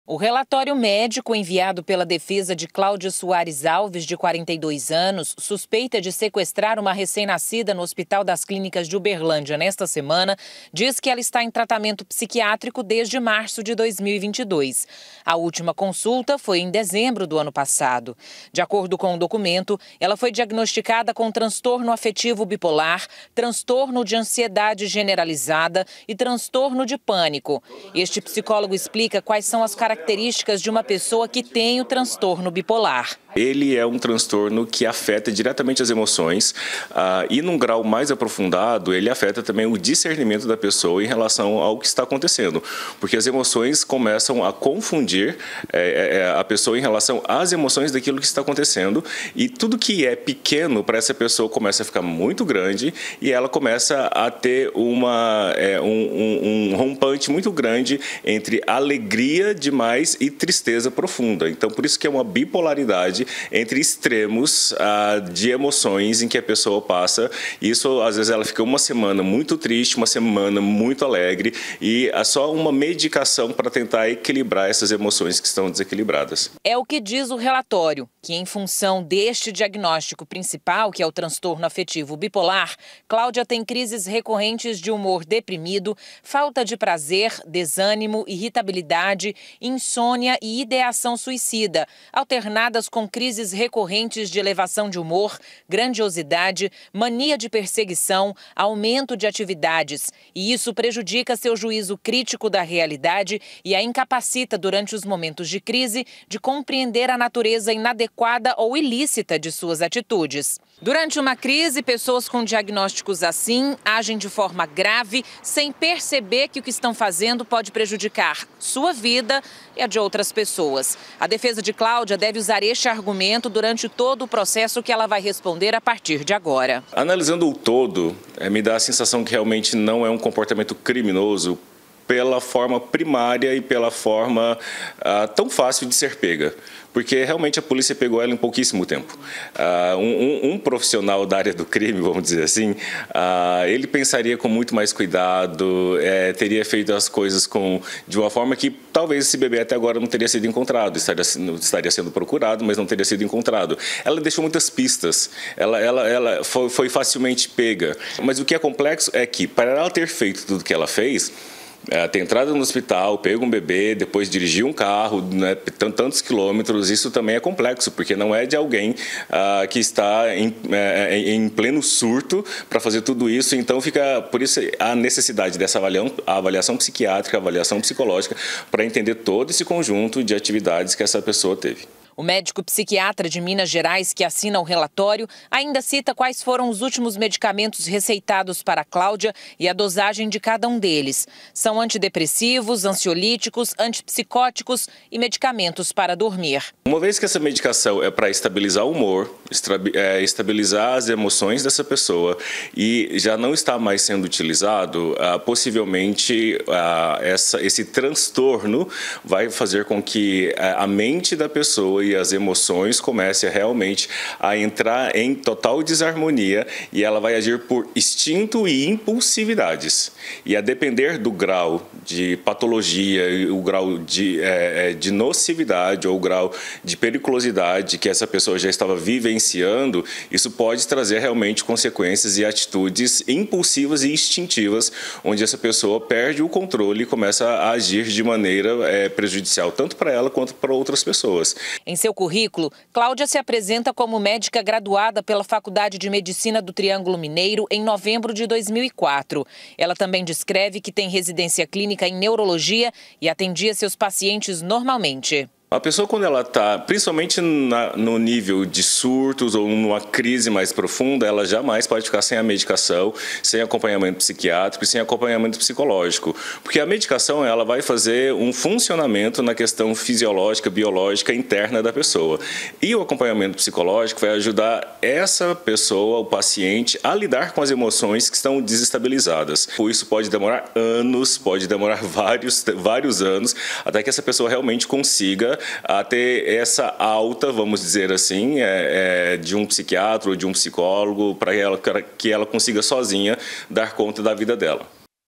SEQUESTRO DO BEBÊ: VOLTAMOS A FALAR SOBRE O CASO DO BEBÊ SEQUESTRADO POR UMA MÉDICA, EM MINAS GERAIS
O QUE LEVA UMA PESSOA BEM SUCEDIDA, RESPEITADA PROFISSIONALMENTE A COMETER UM ATO DESSES? A DEFESA DE CLÁUDIA SOARES ALEGA QUE ELA ESTAVA EM SURTO PROVOCADO POR UM TRANSTORNO NATHALIA MENDONÇA FOI OUVIR UM ESPECIALISTA PARA ENTENDER O QUE PODE TER LEVADO A MÉDICA A SEQUESTRAR A MENINA.